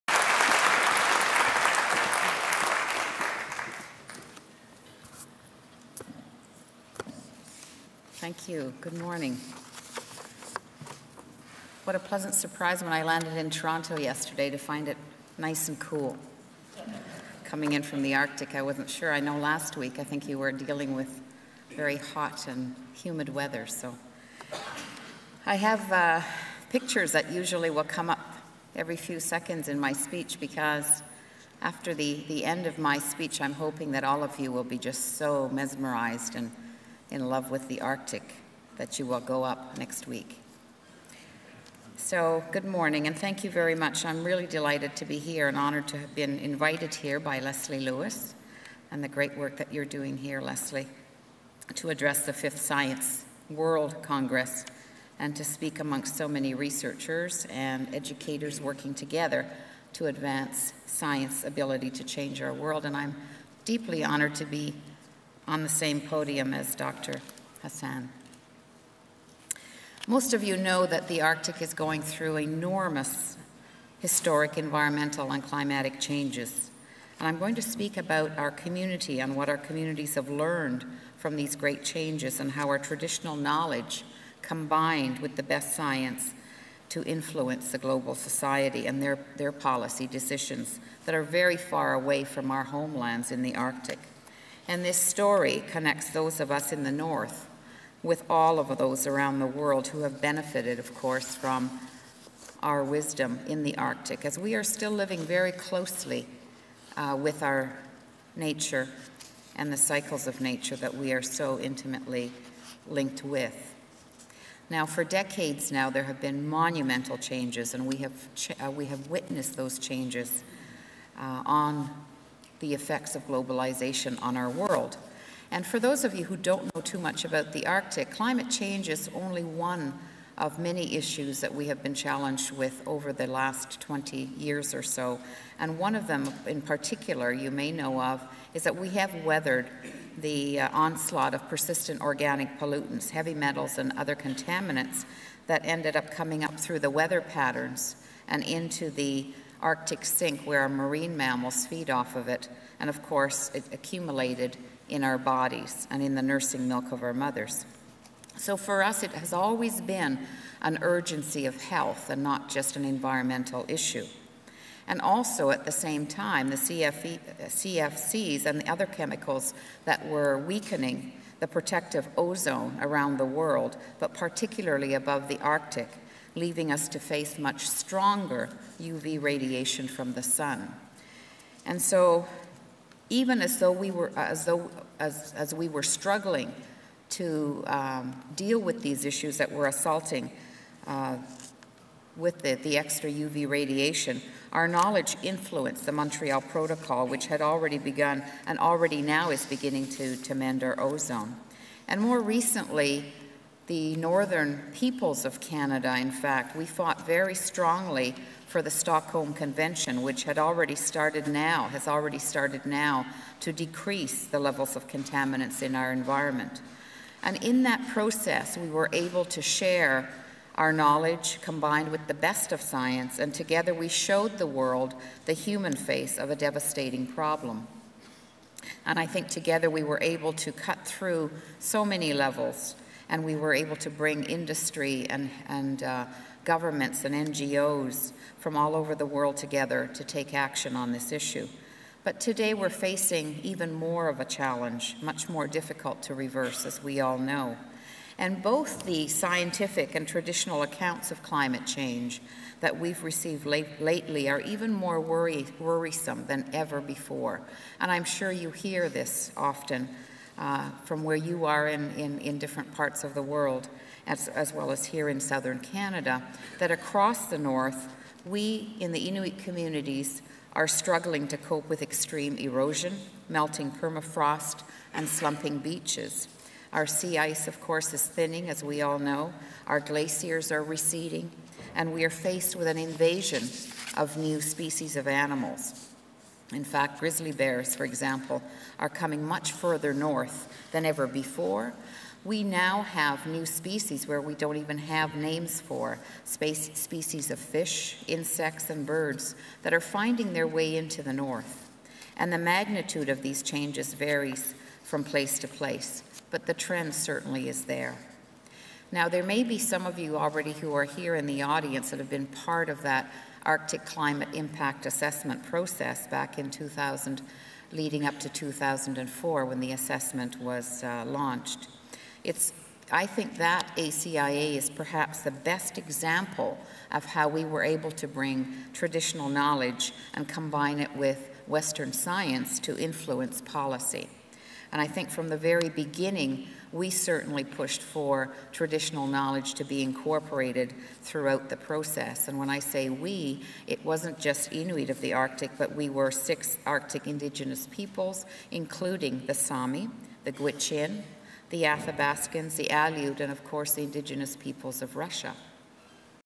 Keynote: Sheila Watt-Cloutier Part 1
Environmental activist Sheila Watt-Cloutier addressed the 5th Science Centre World Congress June 18, 2008.